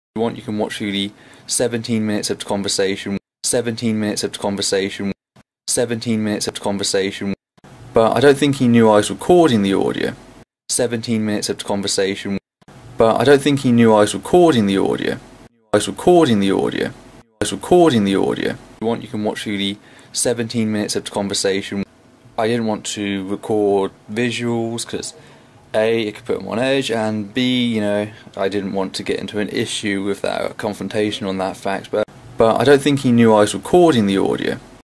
Should EssenceOfThought Apologize?
EssenceOfThought recorded 17 minutes of a conversation with a member of the Christian Union at his school.  He did not inform this person that he was recording, nor get consent for publishing the conversation.  Should EssenceOfThought apologize?